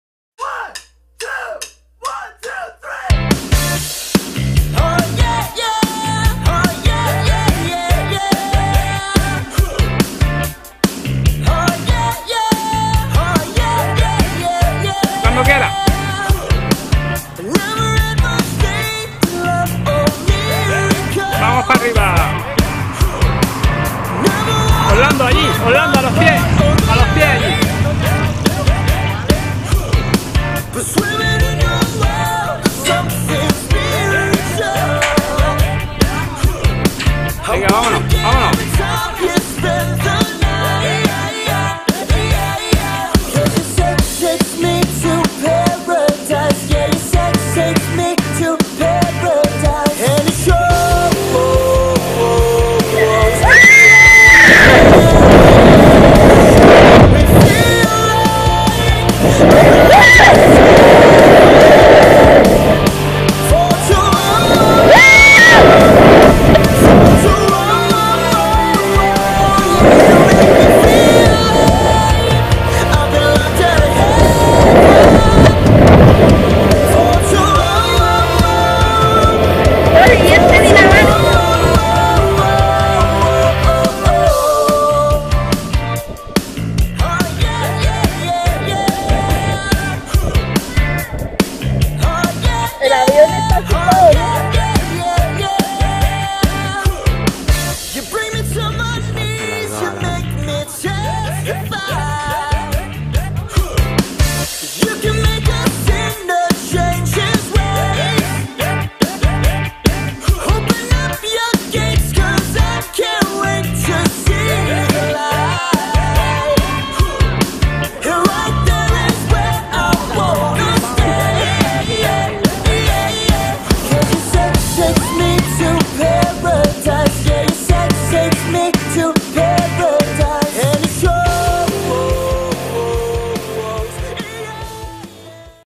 Cora
Esto es hacer puenting en Lanjarón, 90 metros de altura, el salto practicable más alto de Andalucia